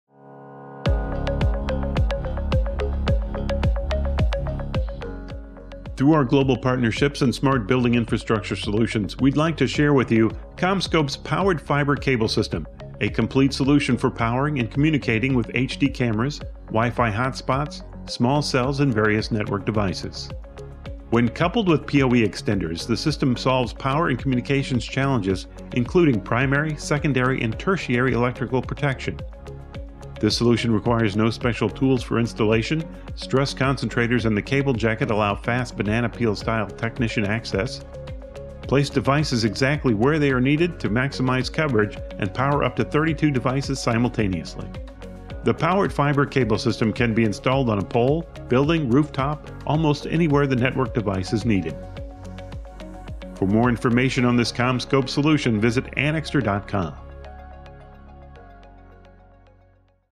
CommScope Powered Fiber Cable System - Anixter Featured Technology
Have 60 seconds? Learn about CommScope's FiberGuide®, an overhead fiber cable management and raceway solution.